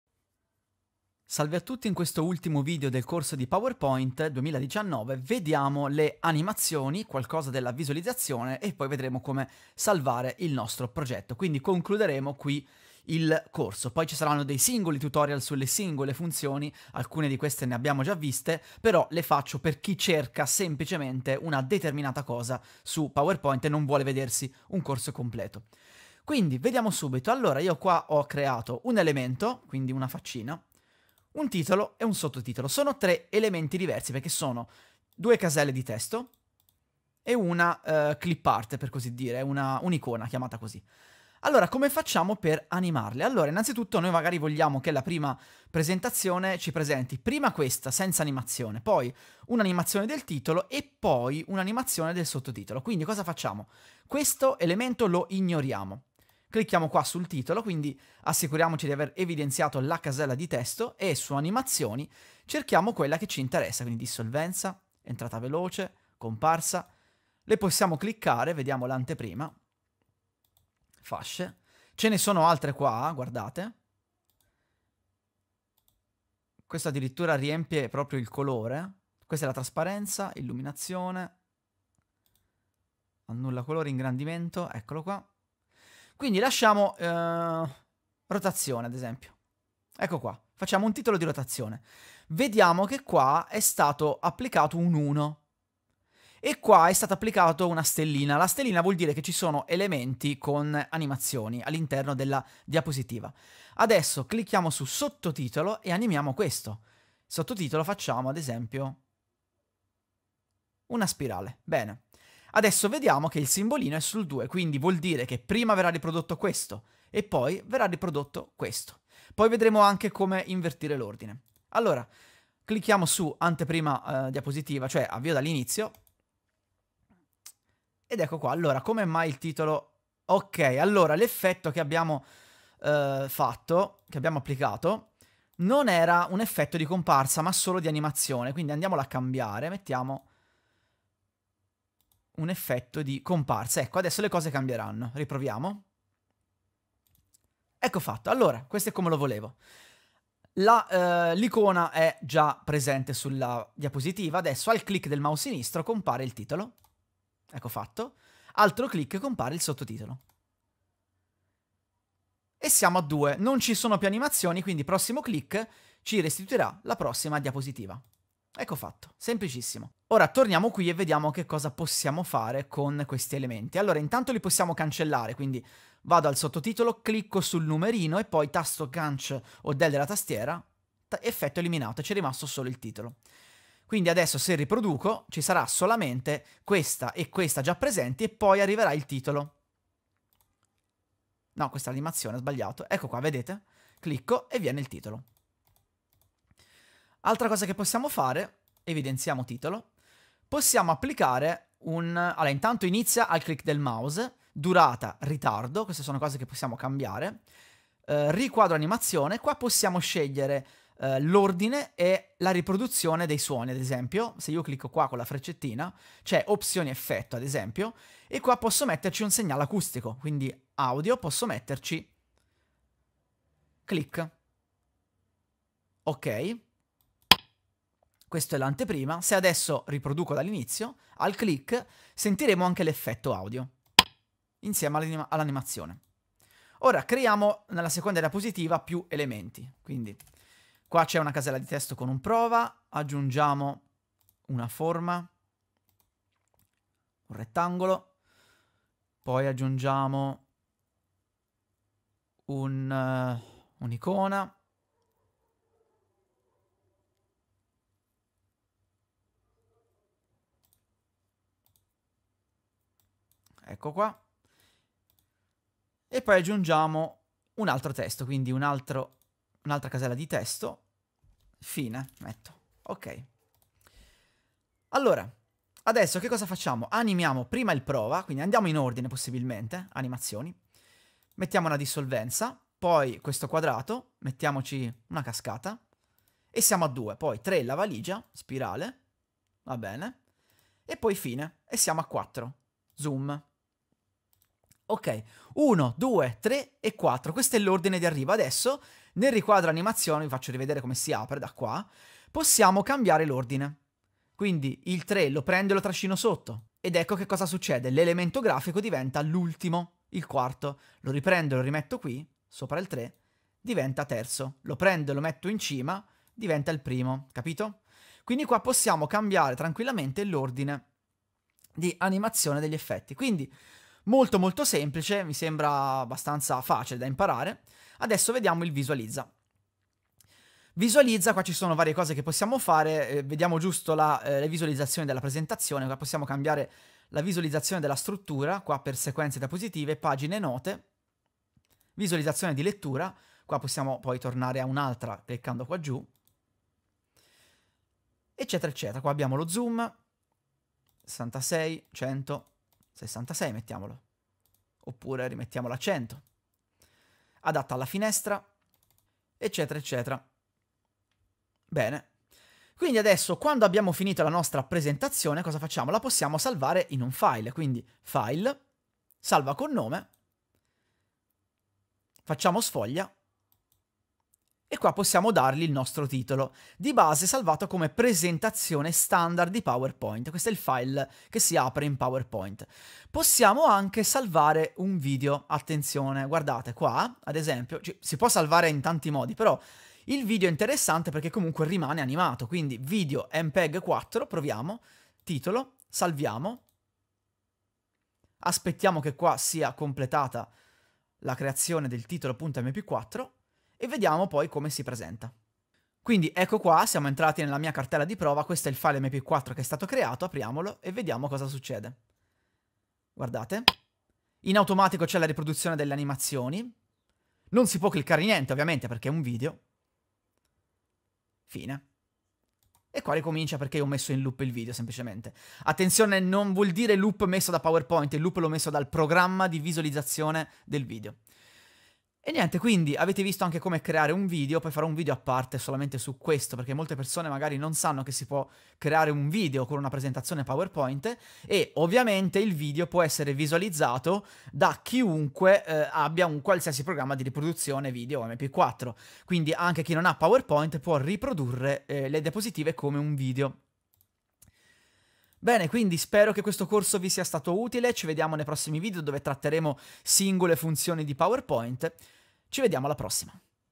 Corso di PowerPoint 2019 - Parte 4 - Animazioni, visualizzazione e salvataggio
In questa ultima parte del corso di PowerPoint 2019 vediamo le animazioni e come possiamo personalizzarle. Vedremo anche la scheda visualizzazione e come salvare il nostro progetto.